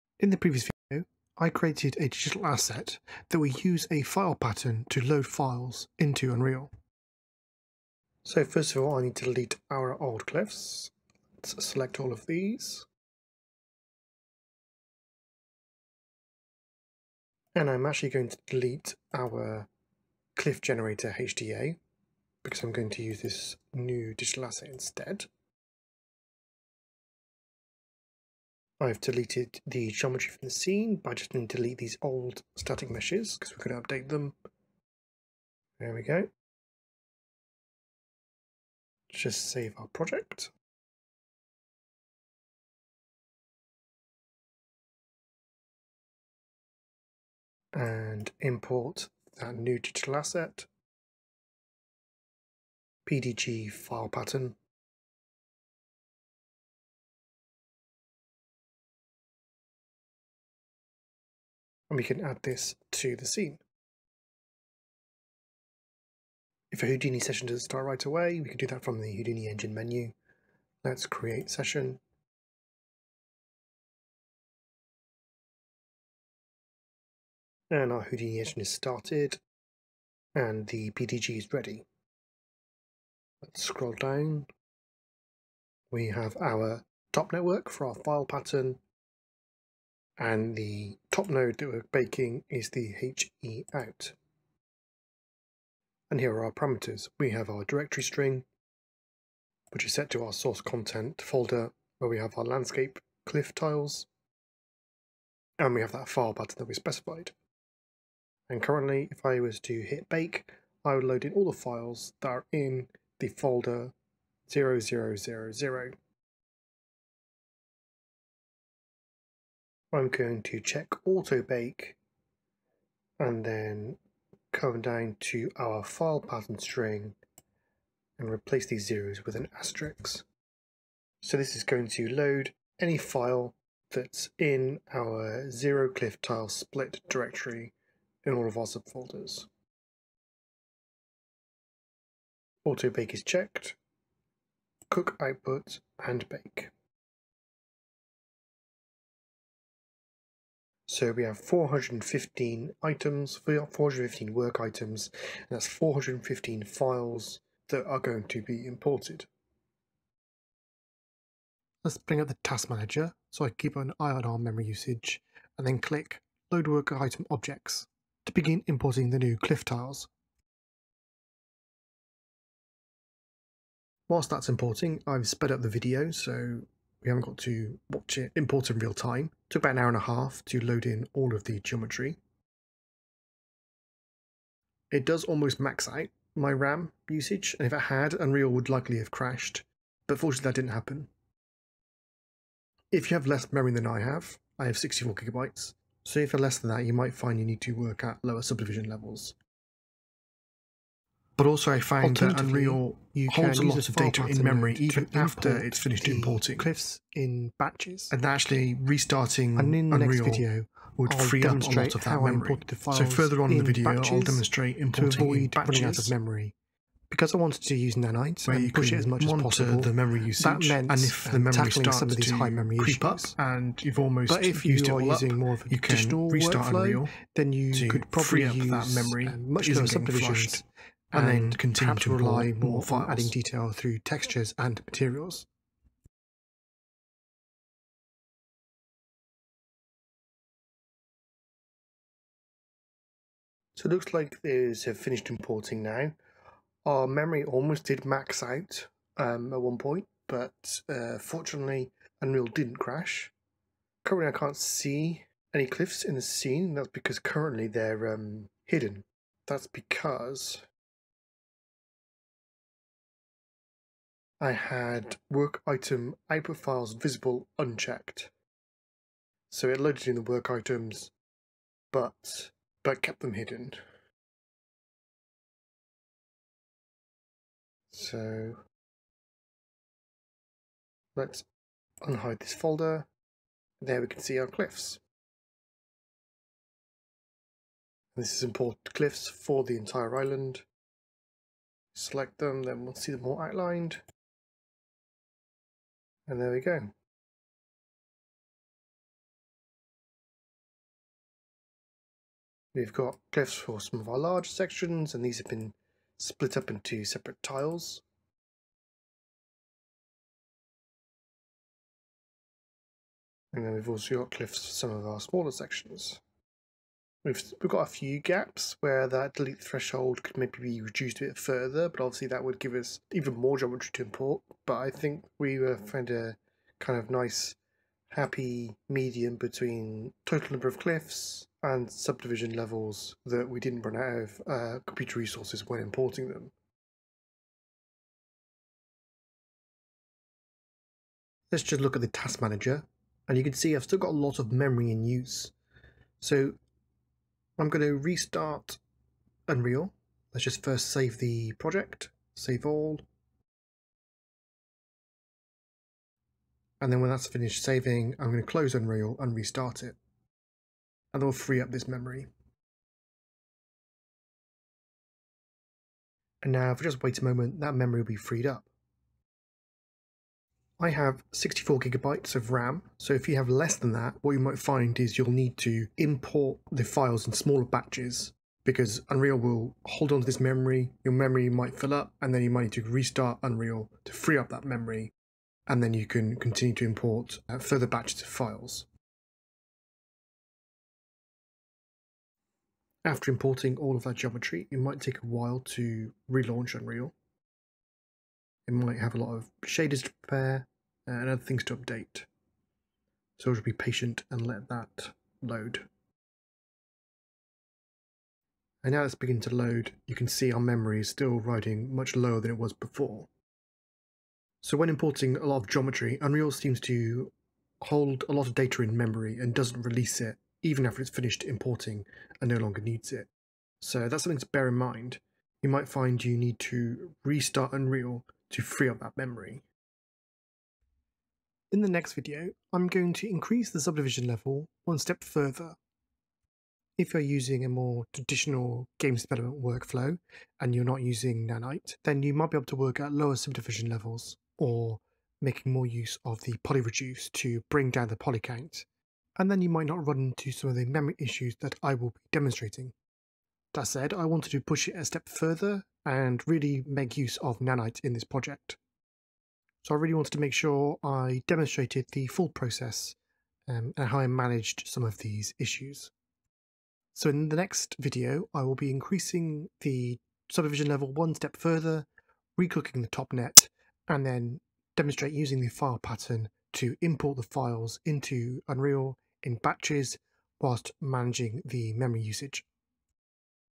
Project Pegasus | Cliff Generation 11 | Importing File Pattern HDA
Learn how to import the File Pattern HDA into Unreal and how it can be used to import the cliff files.